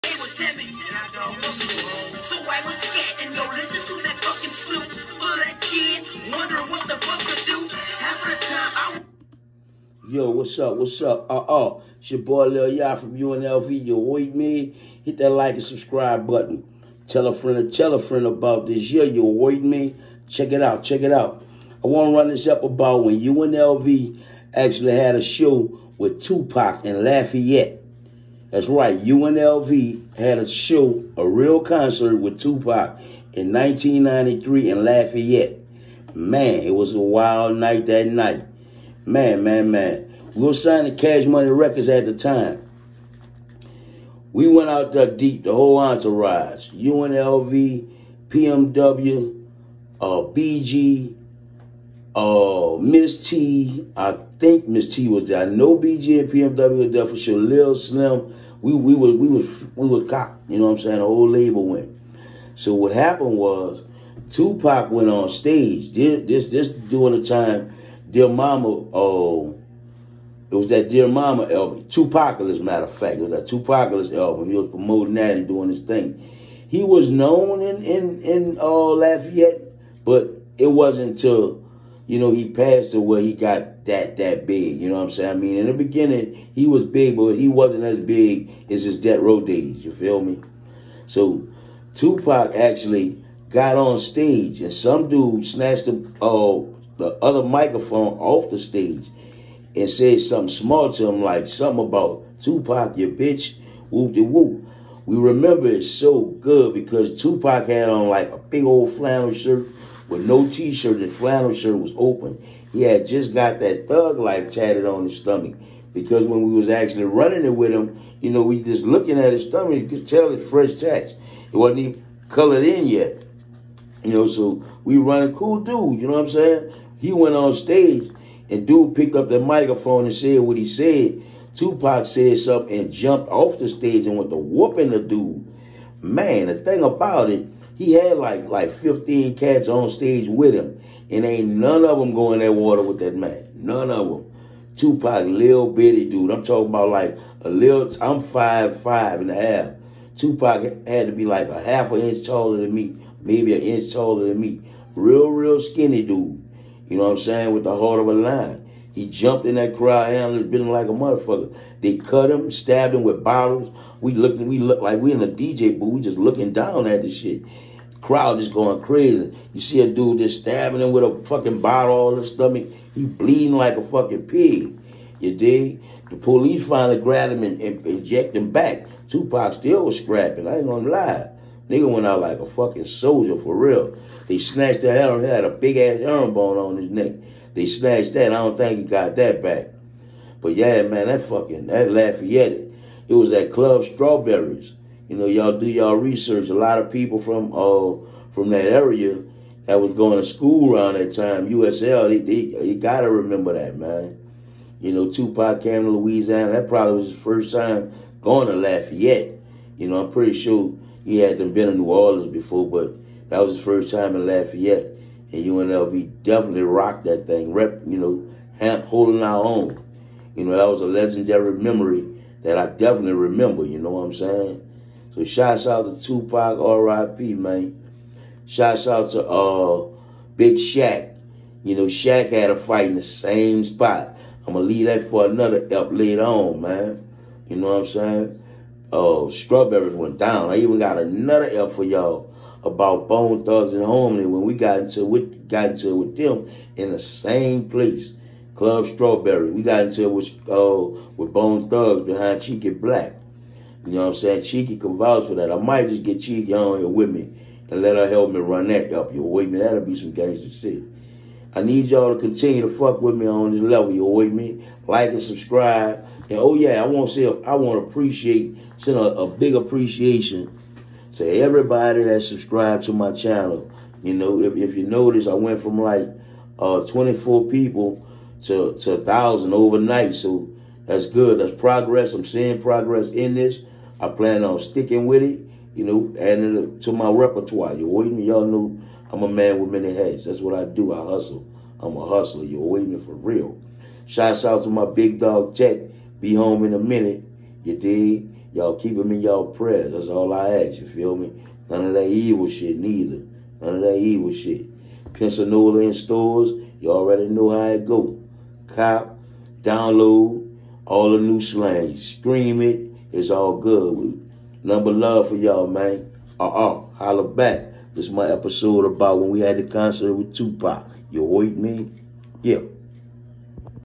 Ep25:UNLV concert with 2Pac when 2Pac gets into a fight and his chain snatched
UNLV concert with 2Pac when 2Pac gets into a fight and his chain snatched